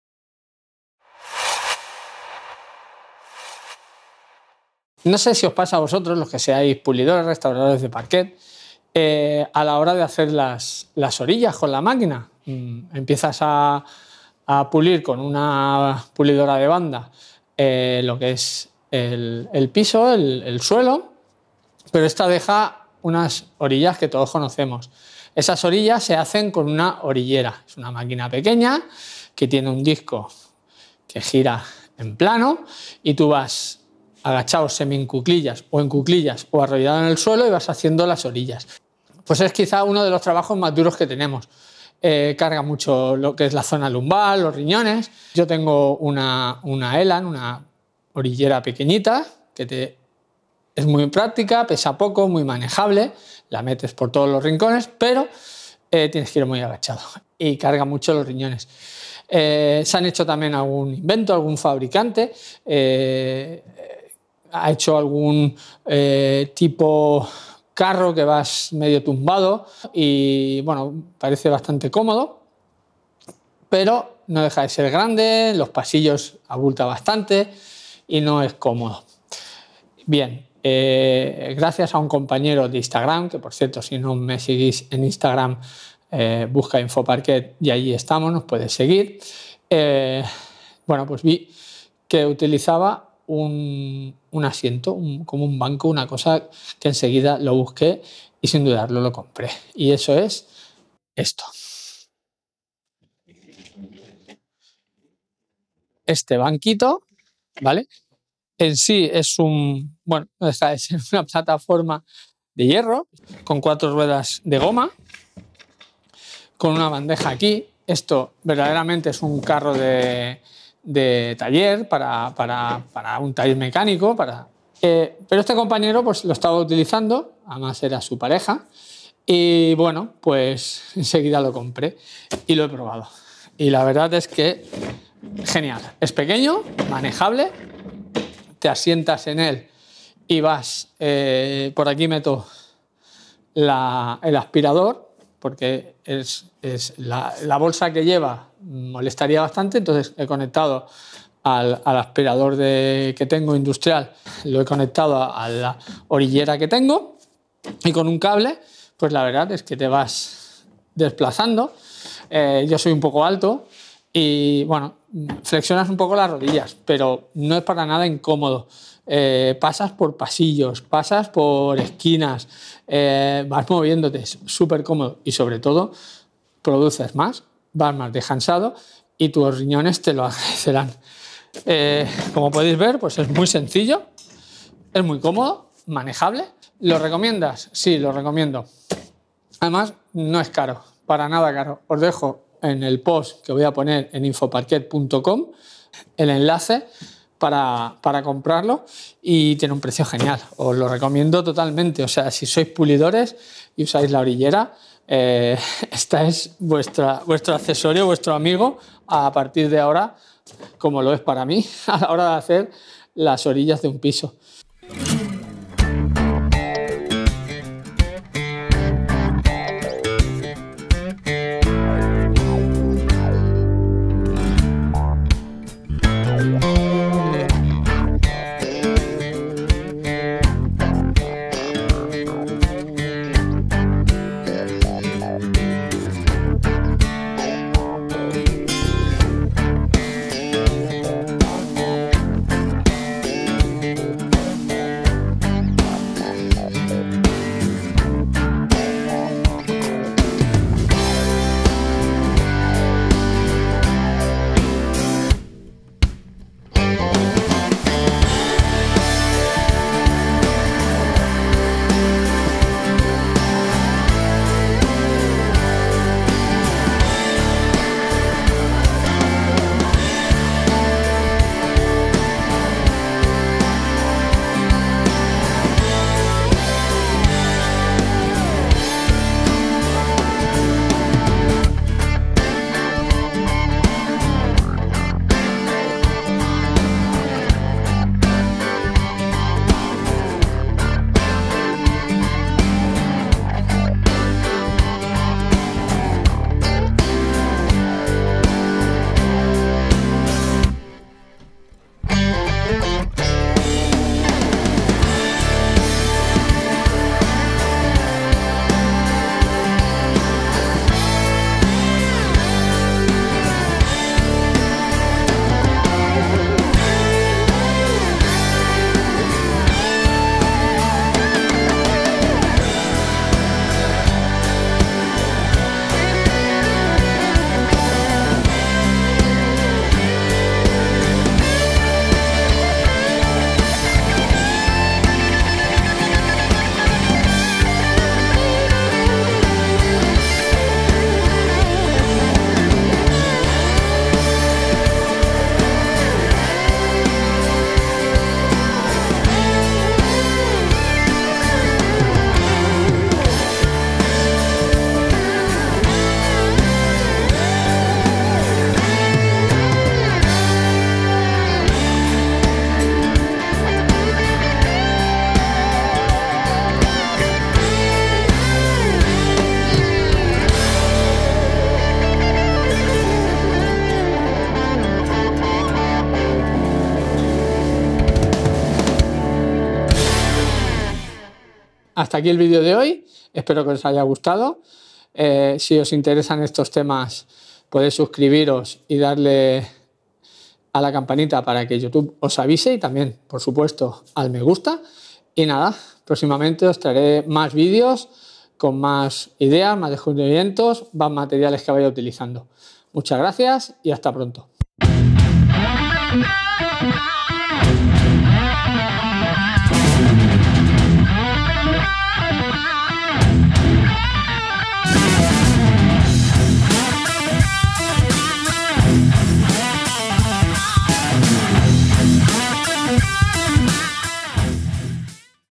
¡No hagas más orillas sin este asiento!
En este video, comparto una solución práctica para uno de los trabajos más duros en la restauración de parquet: el pulido de orillas. Descubre cómo un simple banco con ruedas, originalmente diseñado para talleres mecánicos, puede hacer que el uso de la orillera sea mucho más cómodo y eficiente, aliviando la carga en la zona lumbar y mejorando la productividad. Si eres pulidor o restaurador de parquet, ¡este accesorio puede convertirse en tu mejor aliado!